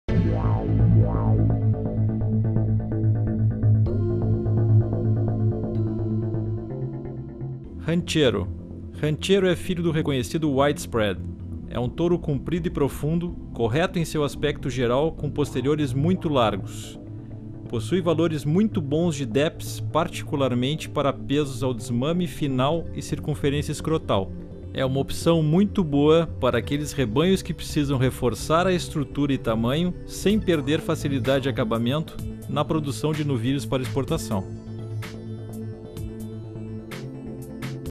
TOURO ANGUS CIALE - Ranchero
Ranchero é filho do reconhecido Widespread. É um touro  comprido e profundo, correto em seu aspecto geral com  posteriores muito largos. Possui valores muito bons de DEPs, particularmente para pesos ao desmame, final e  circunferência escrotal. É uma opção muito boa para aqueles rebanhos  que precisam reforçar a estrutura e tamanho sem perder facilidade de acabamento para produzir novilhos para a exportação.